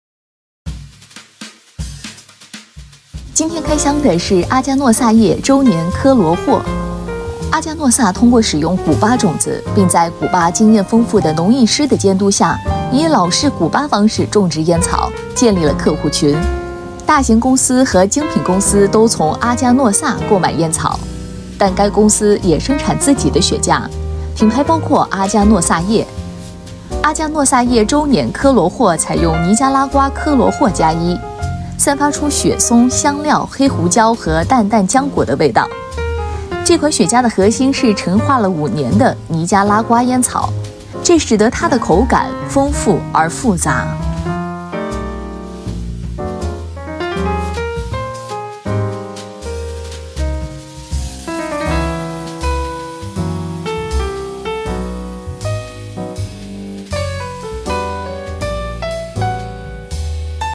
[CoH 雪茄开箱] 紫檀叶周年庆科罗霍
这是一款由著名的尼加拉瓜烟草公司 Aganorsa Leaf（原名 Casa Fernández）推出的特别版雪茄，旨在庆祝公司的重要里程碑或纪念日。Aganorsa Leaf 以其高品质的自种烟叶闻名于世，这些烟叶用于许多备受推崇的雪茄品牌中。
作为一款庆祝性质的雪茄，Aniversario系列往往限量生产，这不仅增加了其收藏价值，也意味着每一支都是经过更加精细的工艺制作而成。